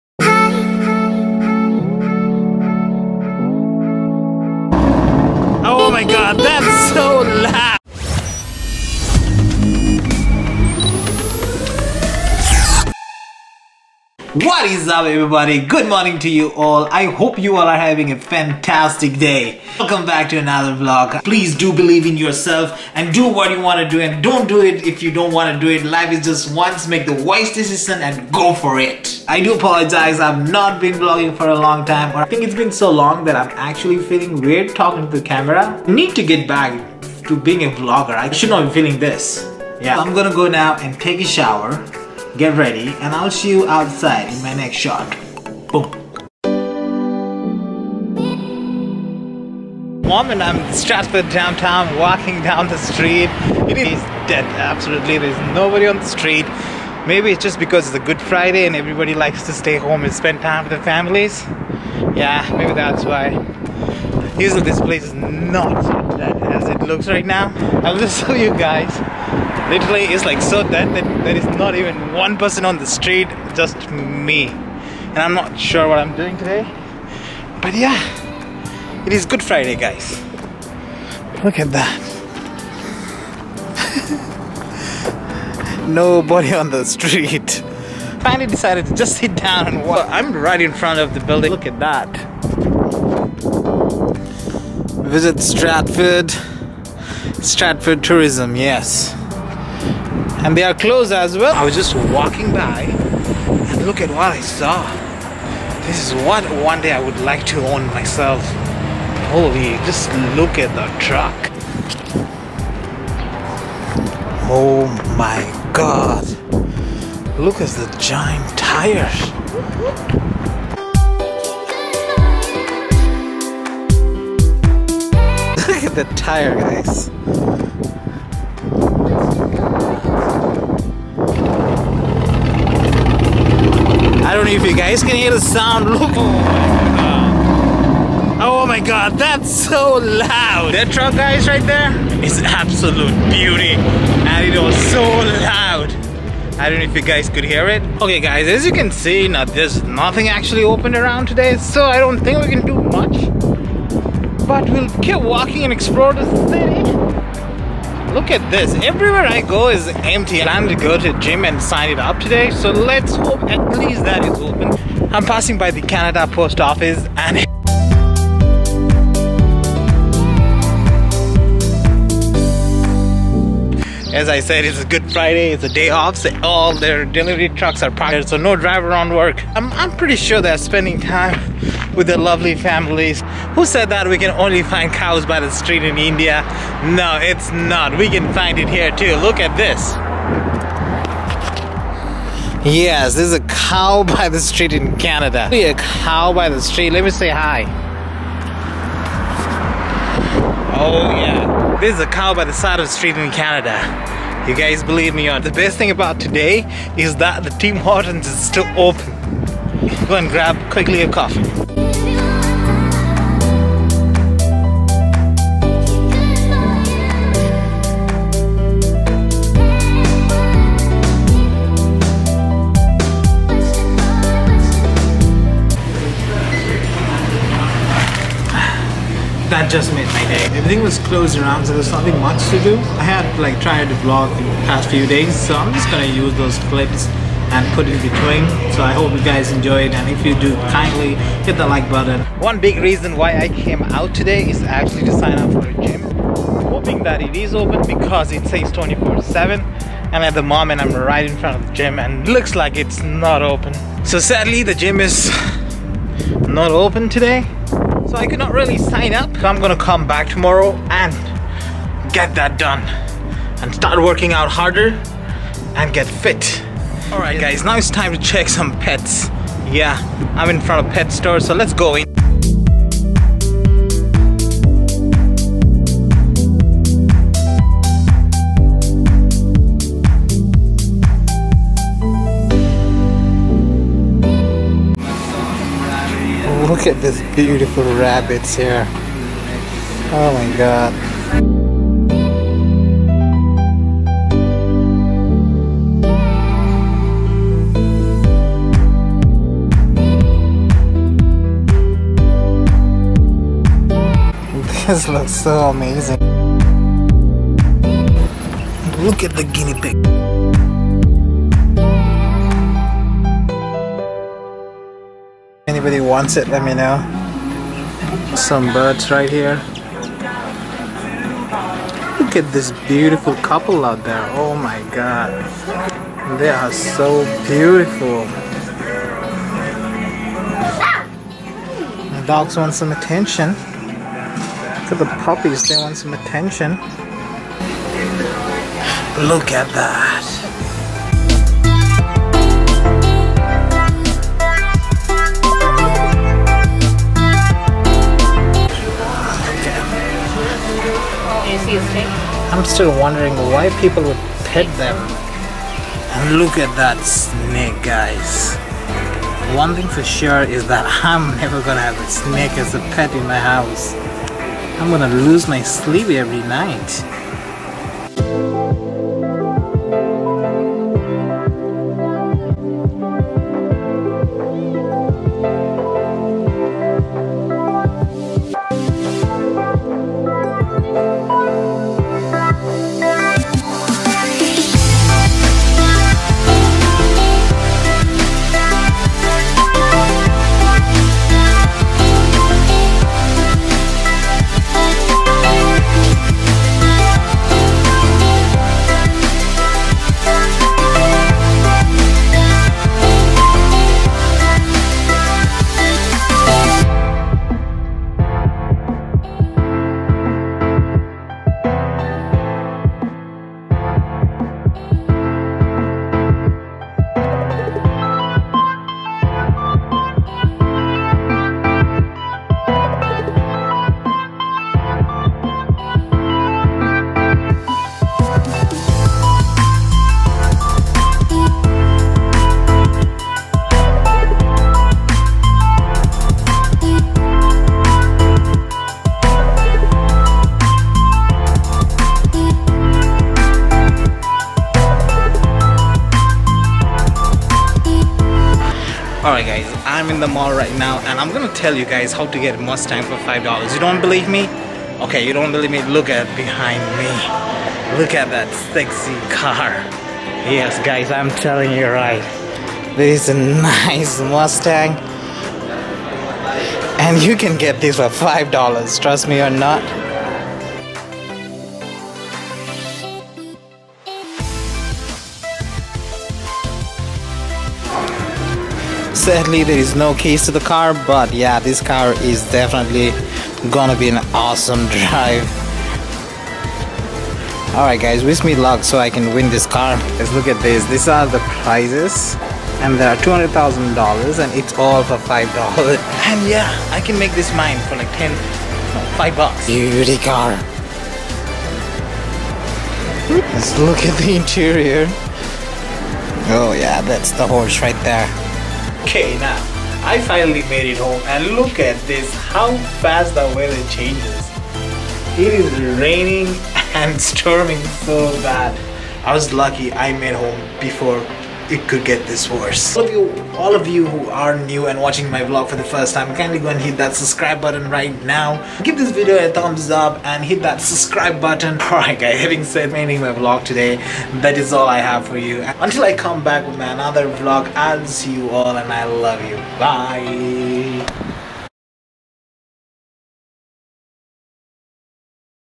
How to get a Ford Mustang for $5
In this Vlogs, I am exploring the beautiful city of Stratford Ontario, on a Good Friday. Due to which everything was closed.
I have included few clips which I had recorded on the past.
I came across on how to win a Ford Mustang with $5 and a giant Truck.
I would like to thank Sunil Sharma, for making the dope bummer for my channel.
I hope you all enjoy . Thank you all